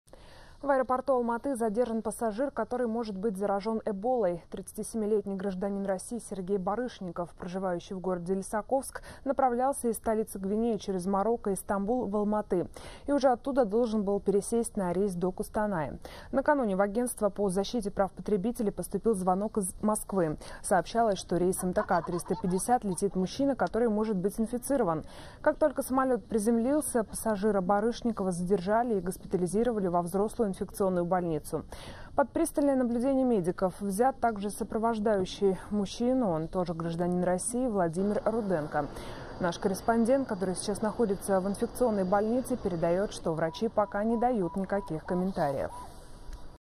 Мужчина с подозрением на Эболу задержан в аэропорту Алматы
Минувшим вечером в аэропорту Алматы после приземления рейса Стамбул - Алматы службами экстренного реагирования был задержан гражданин России. По данным, переданным уполномоченными органами РФ, мужчина может быть заражен вирусом Эбола.